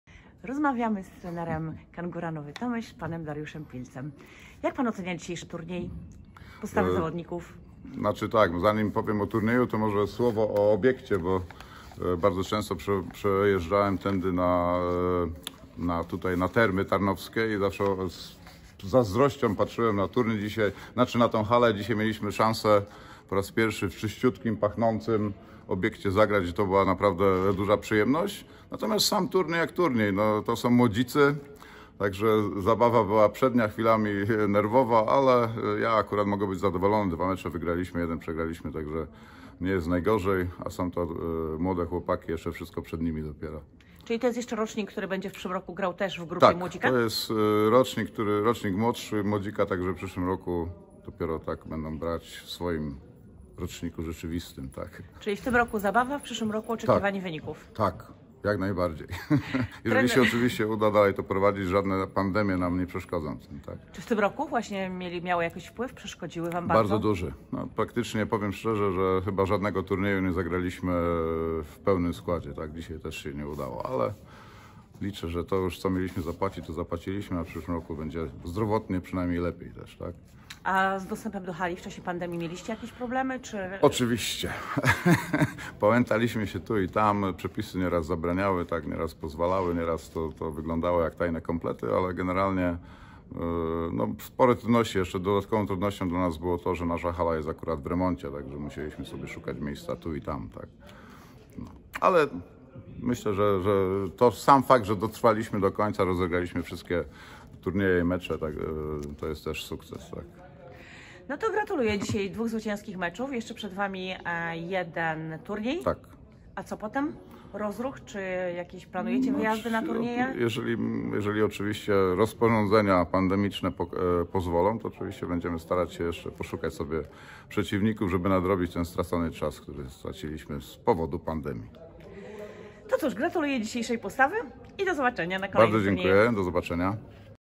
Dariusz Pilz - trener MKS Kangur Nowy Tomyśl
rozmowa po turnieju młodzików w 3 etapie rozgrywek